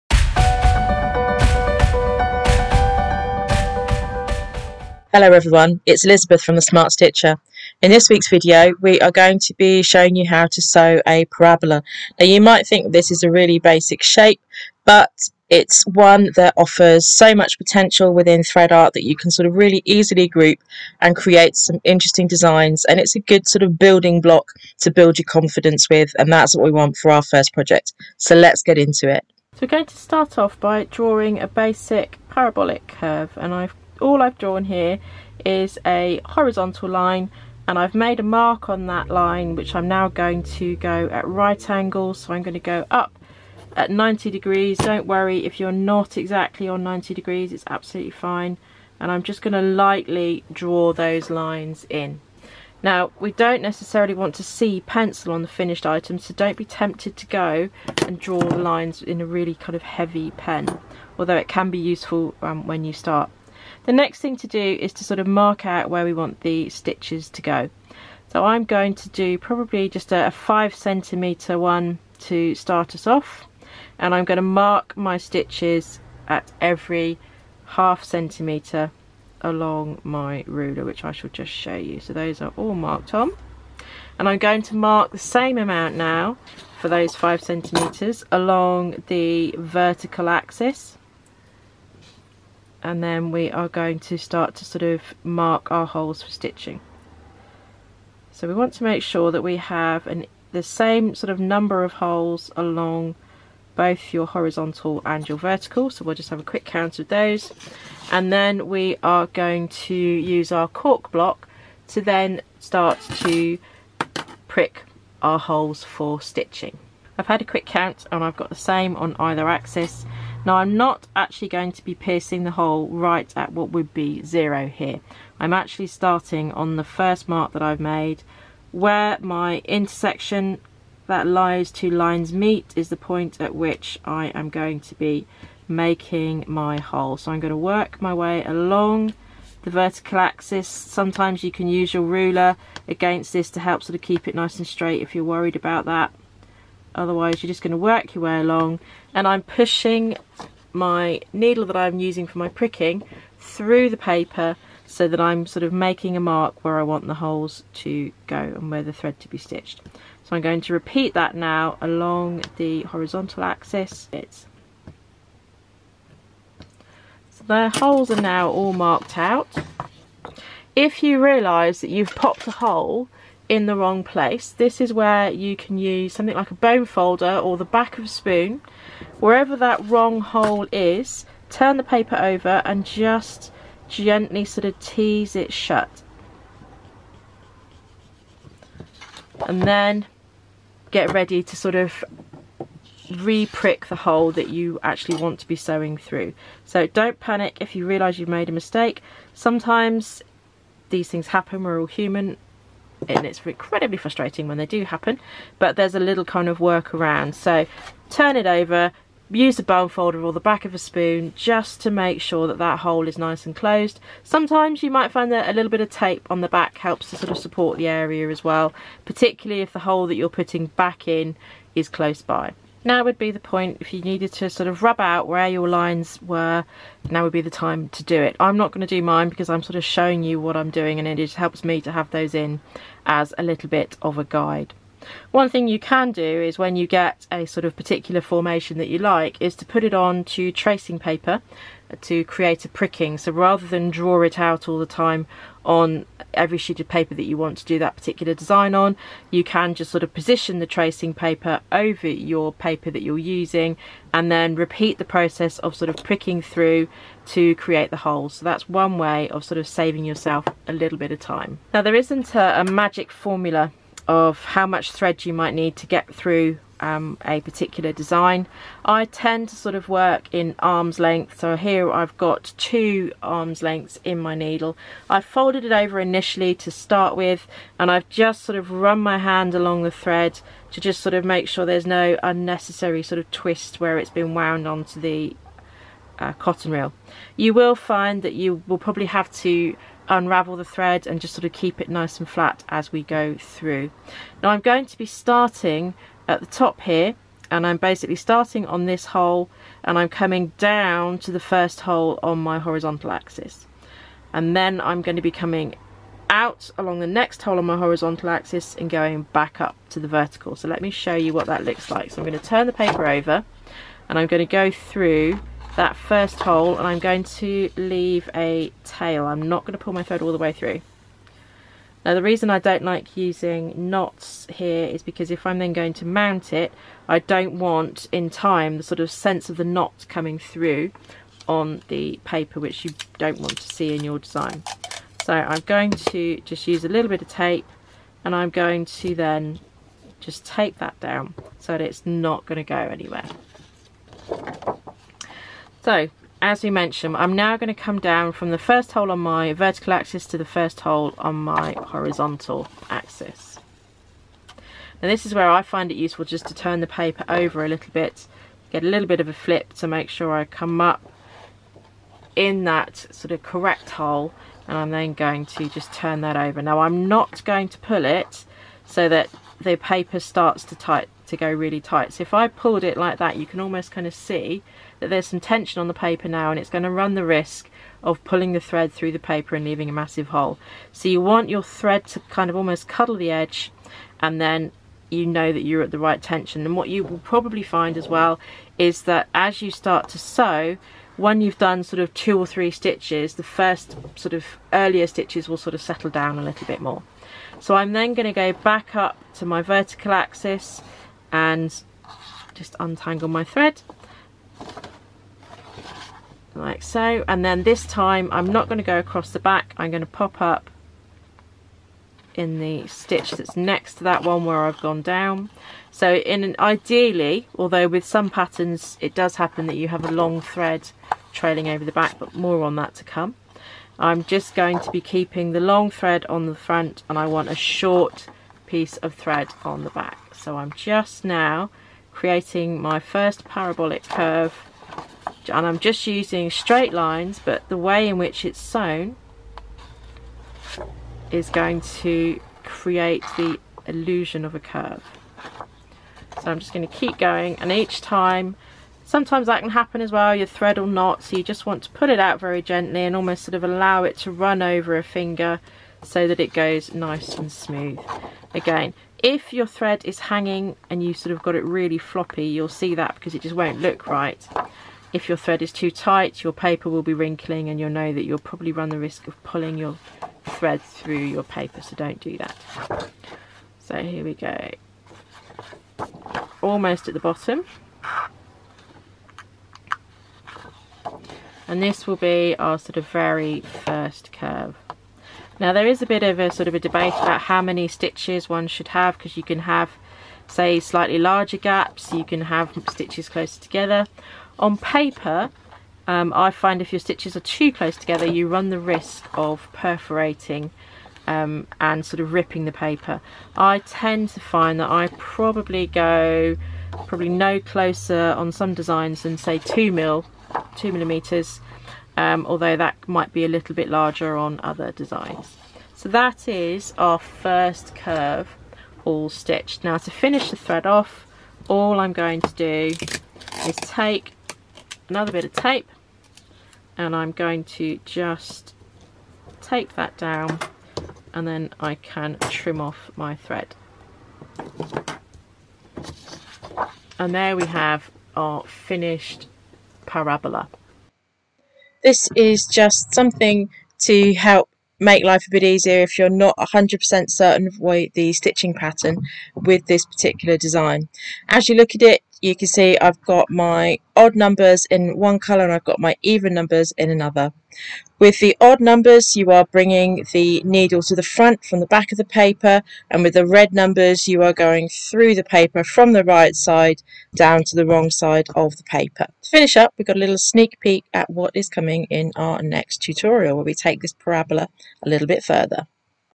How to sew a parabolic curve.
In this clip I show you how to sew a basic parabola, which gives you an excellent foundation for the principles of thread art and the method of sewing used. This may look like a simple design but the possibilities are endless and I show you a few ideas at the end of the clip.

Play about and experiment with combinations of parabolas.

Remember you don't have to keep the axis at right angles either.